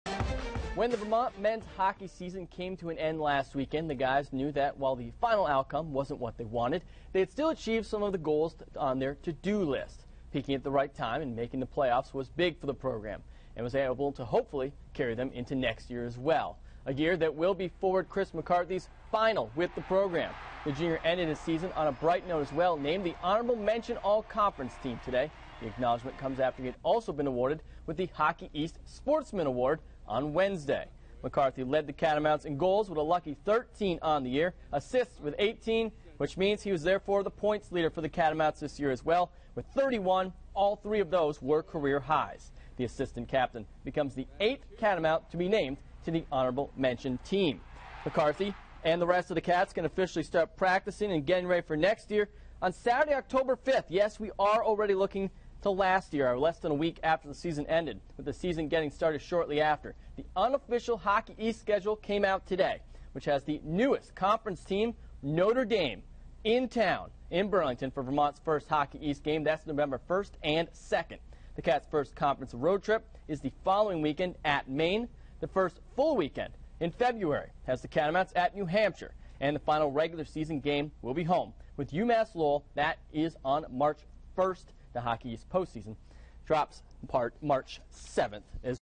UVM men's hockey 2013-14 conference schedule already taking shape
Chris McCarthy named to an all-conference team, and the Hockey East schedule is unofficially out for the UVM men's hockey team